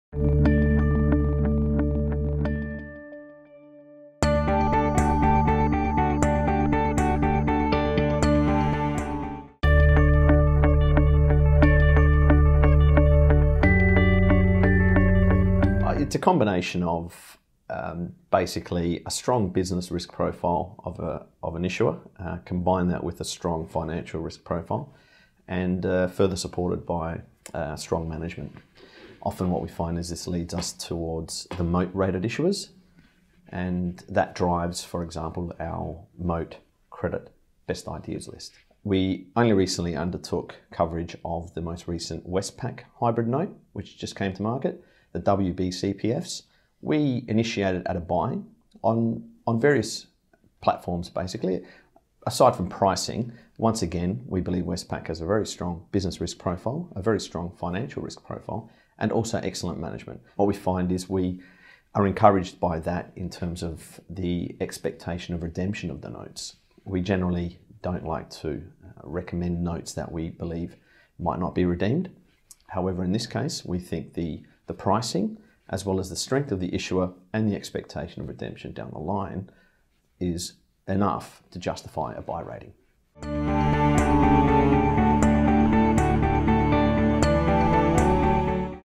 Best Ideas in Credit
Morningstar's John Likos discusses what types of securities are included in the Moat-Focused Credit Best Ideas list.